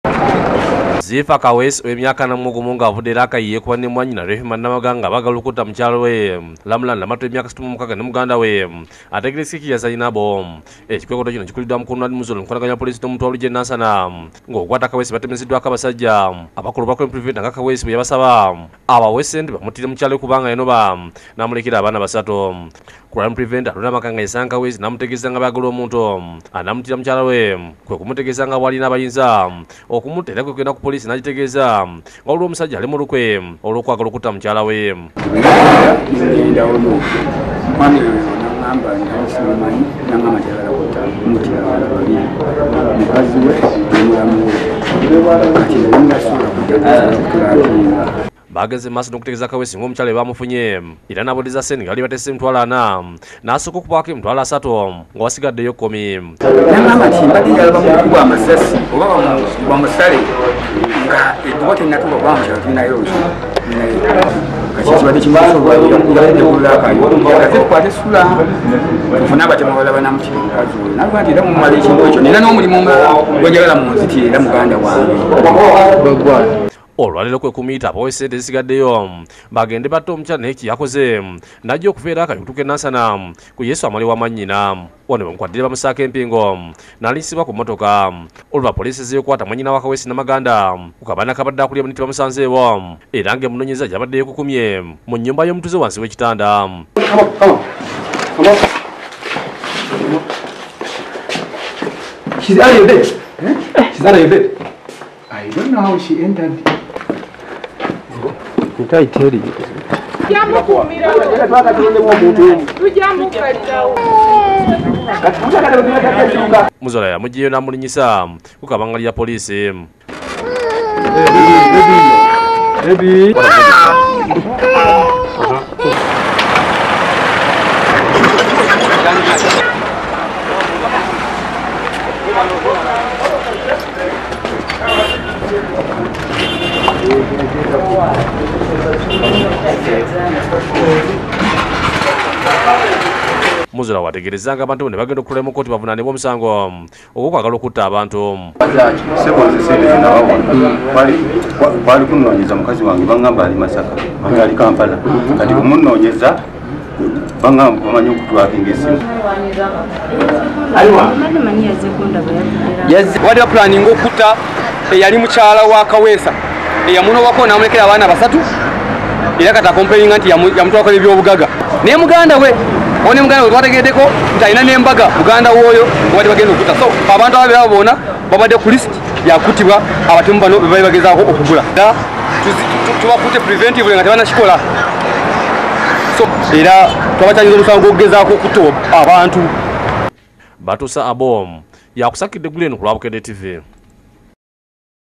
Poliisi ekutte abooluganda babiri
Babalumirizza okugezaako okutemula abakyala babiri.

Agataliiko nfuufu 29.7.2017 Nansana
Reporter: Abu Batuusa